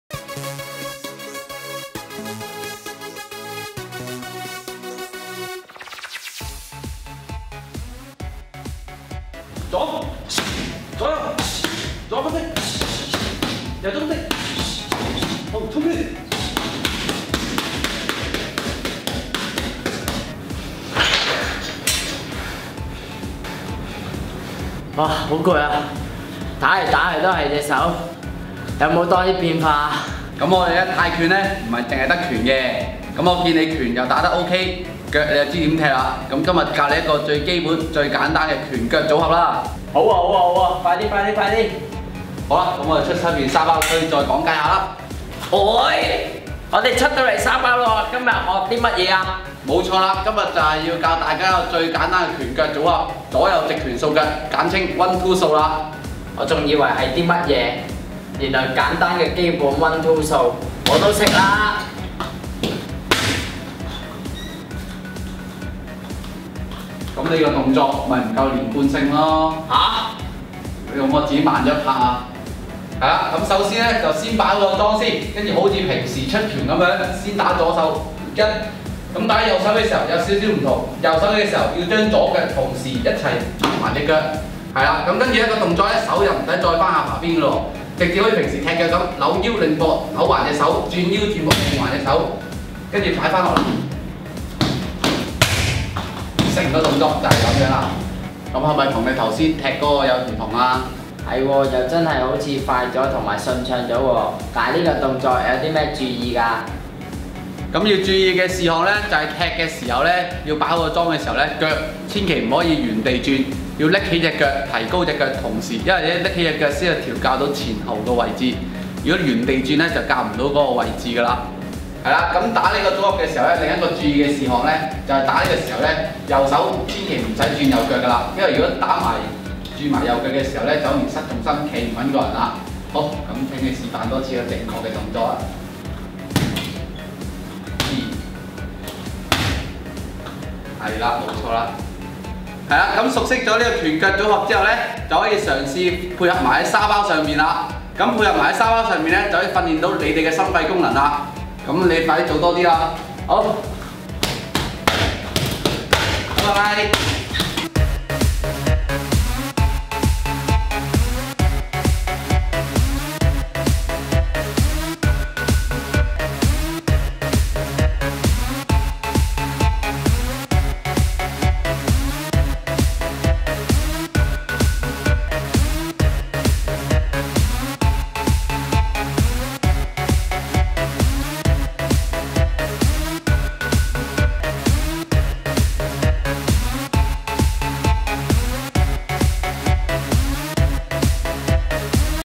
[富德兵工廠] - 27 - 拳手入門 - One Two 掃 - 穆嘉康教練 陳俊賢教練
基本動作掌握之後，我地就可以把動作串連埋一齊
當中有一些小技巧可以令組合連貫及快捷一D
睇下今集的 One...Two...掃... 啦

富德拳館頻道仲有咩睇?

我要做拳手！

我要瘦身、練大隻、增肌肉！

練習前，記住要熱身！